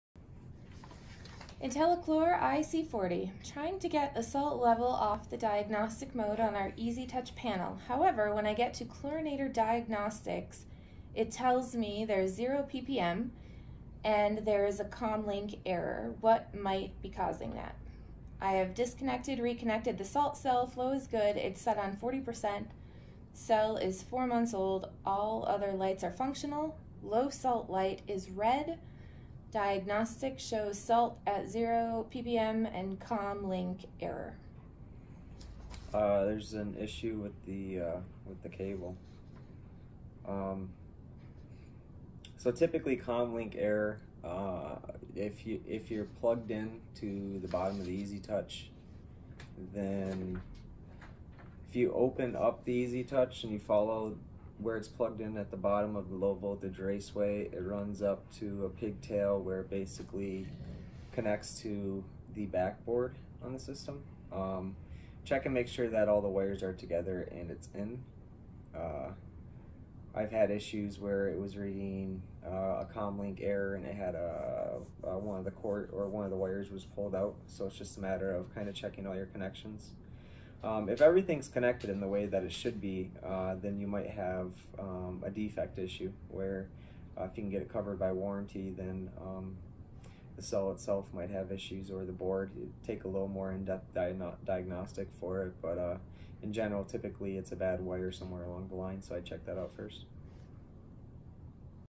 Ask the Pool Guy: intellichlor ic40 comm link error
intelliChlor resources and parts

intelliChlor Salt Generating units by Pentair are a popular option for swimming pools. The cells, when combined with an easyTouch or intelliTouch control center, need to be enabled to tell them to turn on and start working. If the pool is just being opened in the spring, there is a chance even enabled the cells will not work. They are equipped with a temperature sensor, which won’t work when the water is too cold, below 52 degrees F it turns off. Once it is above that range, it will begin to make your chlorine.  Enable, and then monitor the cell and water temp so it will kick on and start making chlorine when it reaches the right temperature.